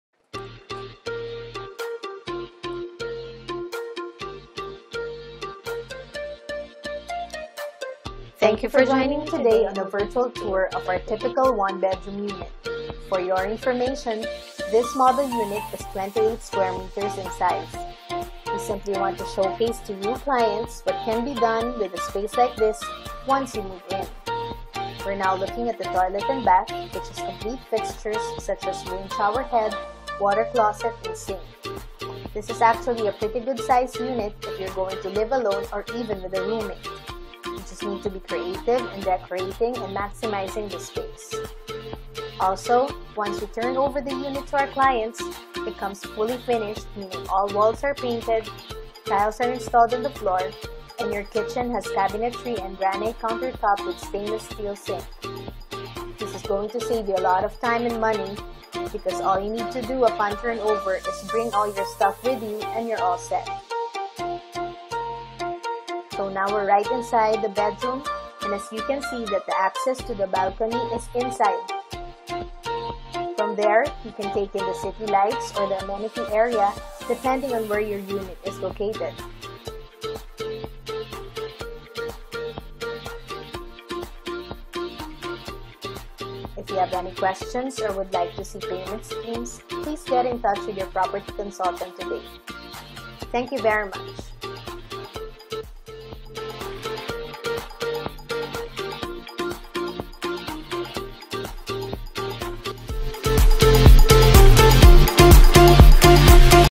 DMCI Homes 1BR Model Unit Tour
This video is a virtual tour of our typical 1BR Model Units in DMCI Homes.

✔Resort inspired amenities
✔Best for Airbnb / Transient business
✔High rental potential
✔High resale value
✔NO SPOT CASHOUT
✔Cheaper price per sqm in the area